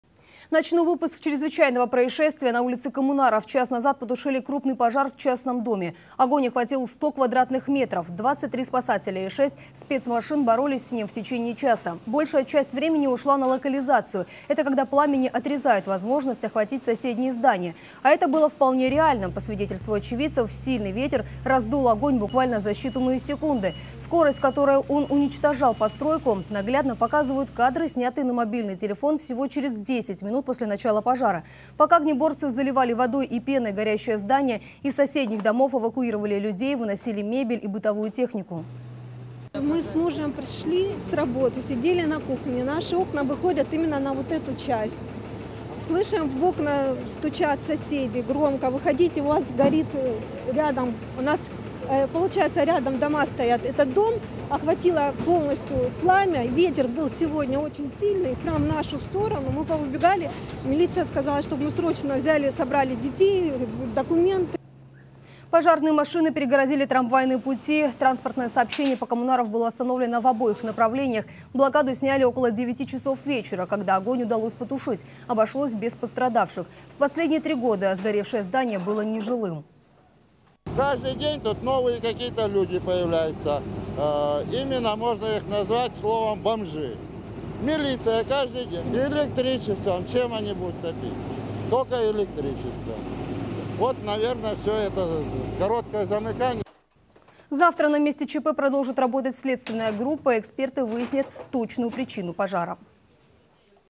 В центре Краснодара потушили пожар на площади 100 кв. м
На улице Коммунаров в Краснодаре 3 февраля около 21.00 потушили крупный пожар в частном доме.